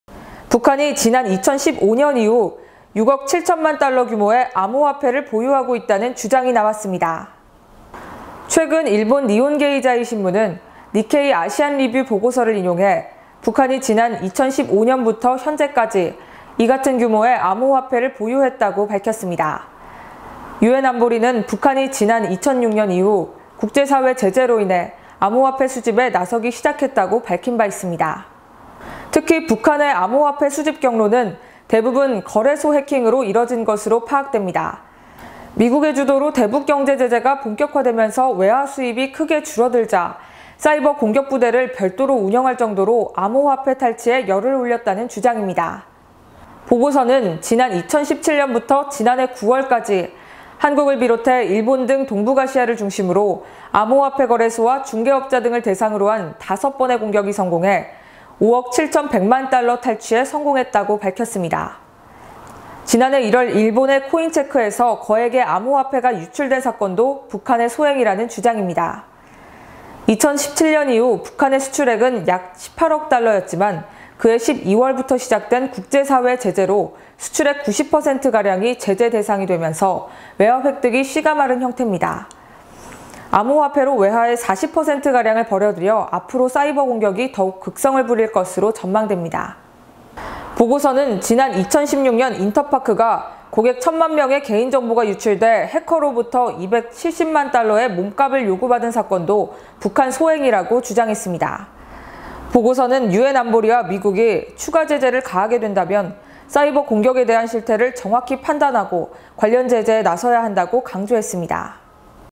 [라이브썰] 北, 암호화폐 탈취로 외화 충당 … 韓 거래소 타깃
북한이 지난 2015년 이후 6억7000만 달러(약 7591억 원) 규모의 암호화폐를 보유하고 있다는 주장이 나왔습니다.

최근 일본 니혼게이자신문은 닛케이 아시안리뷰 보고서를 인용해 북한이 지난 2015년부터 현재까지 이같은 규모의 암호화폐를 보유했다고 밝혔습니다. 유엔안보리는 북한이 지난 2006년 이후 국제사회 제재로 인해 암호화폐 수집에 나서기 시작했다고 밝힌 바 있습니다.

특히 북한이 암호화폐 수집 경로는 대부분 거래소 해킹 등으로 이뤄진 것으로 파악됩니다. 미국의 주도로 대북 경제 제재가 본격화되면서 외화 수입이 크게 줄어들자 사이버 공격 부대를 별도로 운영할 정도로 암호화폐 탈취에 열을 올렸다는 주장입니다.

보고서는 지난 2017년부터 지난해 9월까지 한국을 비롯해 일본 등 동북아시아를 중심으로 암호화폐 거래소와 중개업자 등을 대상으로 한 5번의 공격이 성공해 5억7100만 달러(약 6492억 원) 탈취에 성공했다고 밝혔습니다.

지난해 1월 일본의 코인체크에서 거액의 암호화폐가 유출된 사건도 북한의 소행이라는 주장입니다. 2017년 기준 북한의 수출액은 약 18억 달러(약 2조466억 원)였지만 그해 12월부터 시작된 국제사회 제재로 수출액 90%가량이 제재 대상이 되면서 외화 획득이 씨가 마른 형태입니다. 암호화폐로 외화의 40%가량을 벌어들여 앞으로 사이버 공격이 더욱 극성을 부릴 것으로 전망됩니다.

보고서는 지난 2016년 인터파크가 고객 1000만 명의 개인정보가 유출돼 해커로부터 270만 달러의 몸값을 요구받은 사건도 북한 소행이라고 주장했습니다. 보고서는 유엔안보리와 미국이 추가 제재를 가하게 된다면 사이버 공격에 대한 실태를 정확히 판단하고 관련 제재에 나서야 한다고 강조했습니다.